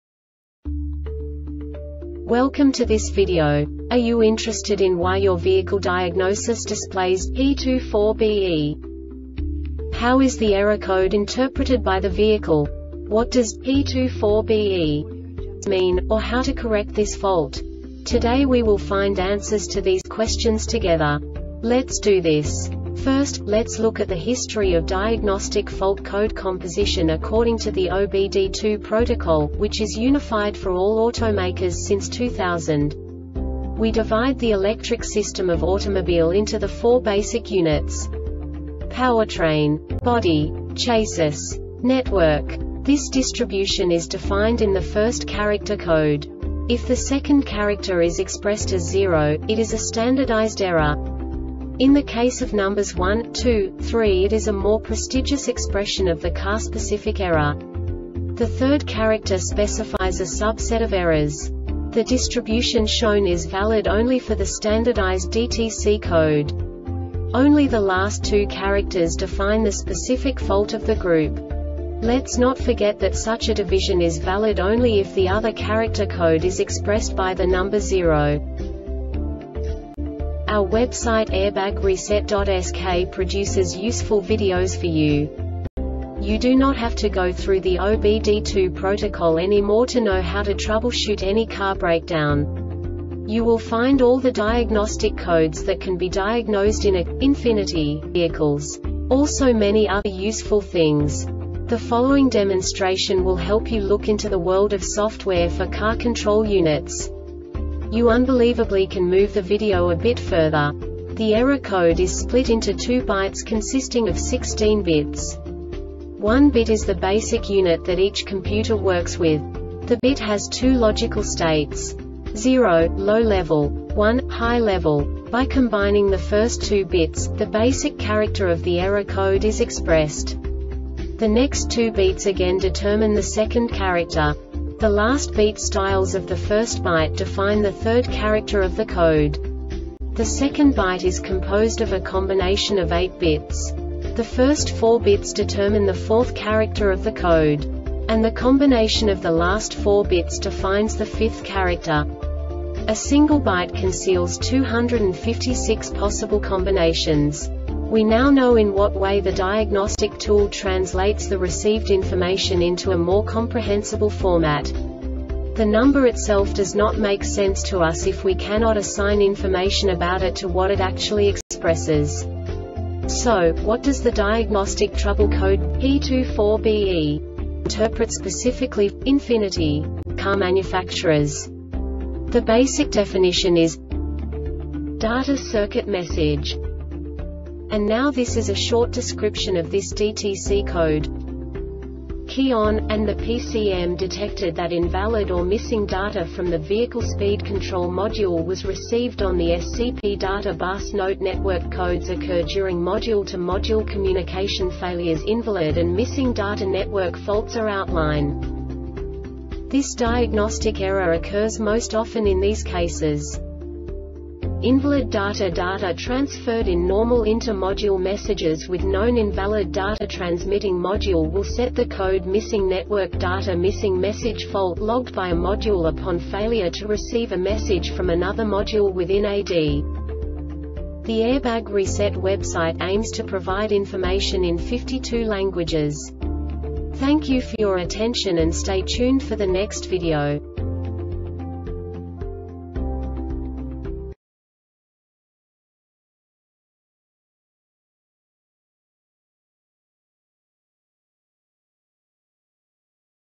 DTC Ford P24BE Short Explanation
Contents:
0:21 Basic DTC analysis according to OBD2 protocol standard.
1:48 Insight into programming
2:58 A diagnostically understandable description of the Diagnostic Trouble Code
3:05 Basic error definition

Make:
Ford
Code:
P24BE
Definition:
Data Circuit Message
Description:
Key on,and the PCM detected that invalid or Missing Data from the Vehicle Speed Control Module was received on the SCP data bus.Note: Network codes occur during module-to-module communication failures.Invalid and Missing data network faults are outline
Cause: